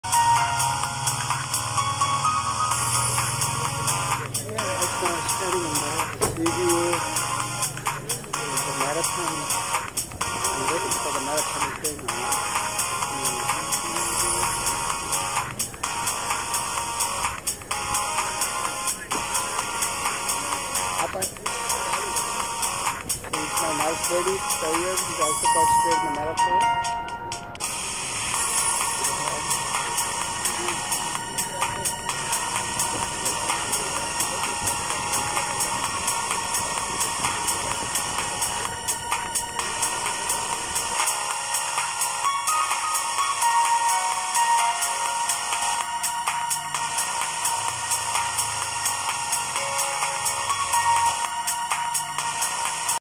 Marathon 2024
Azhar with Commissioner Karachi marathon took place at Seaview